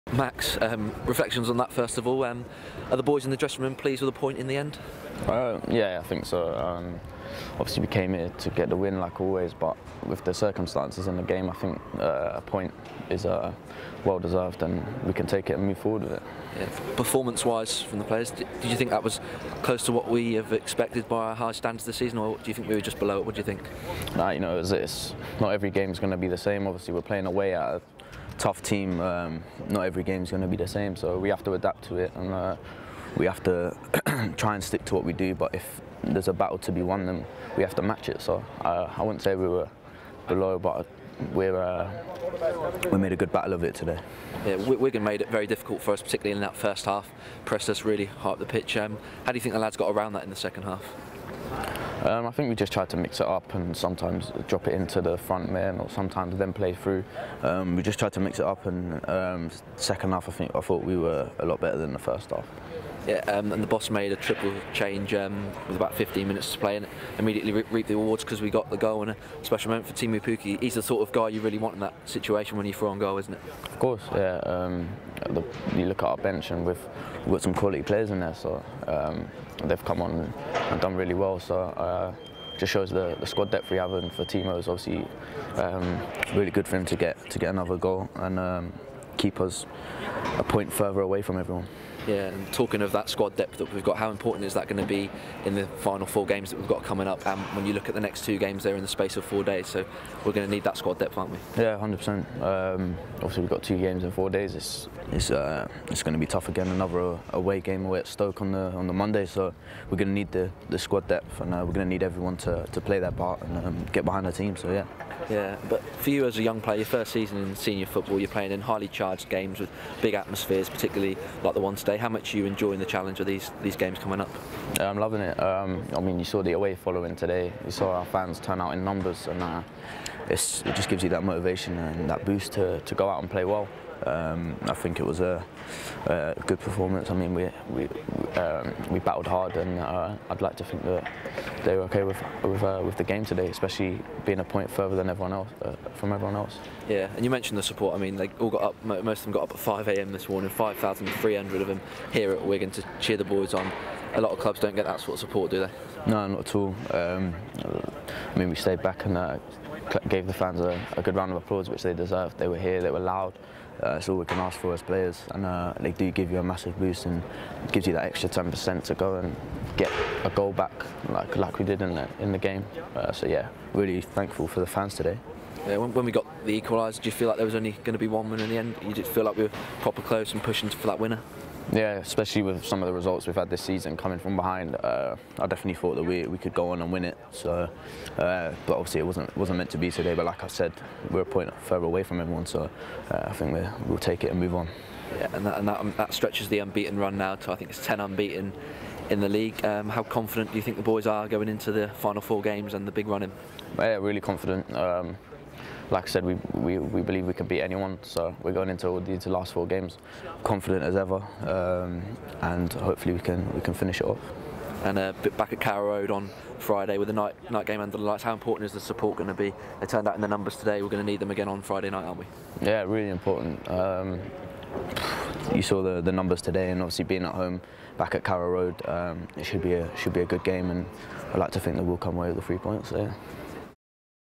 Wigan Athletic 1-1 Norwich City: Max Aarons Reaction
Norwich City right back, Max Aarons reflects on City's 1-1 draw with Wigan Athletic on Saturday 14th April 2019.